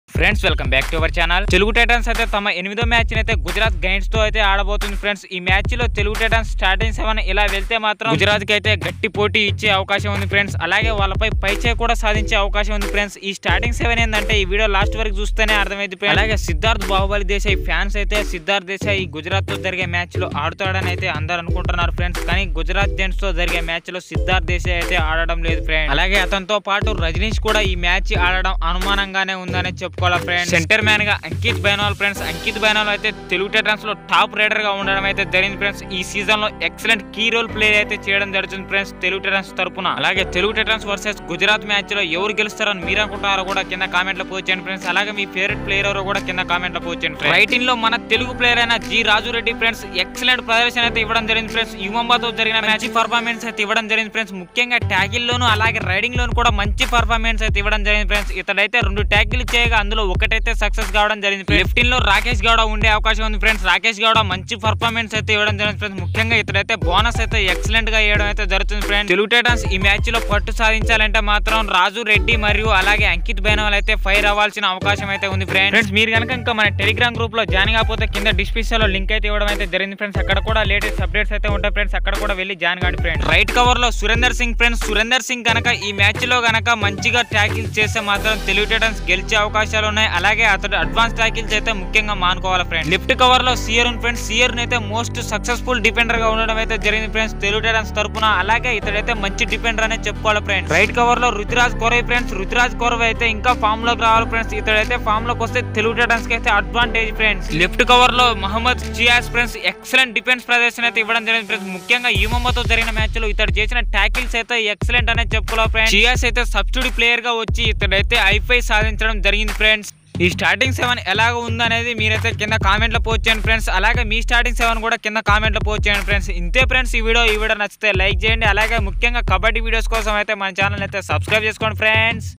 TELUGU TITANS BEST STARTING 7 AGAINST GUJARAT GIANTS IN TELUGU GUJARAT GIANTS VS TELUGU TITANS PKL8
telugutitans
telugutitans
prokabaddi
telugutitans

Least sports update on this group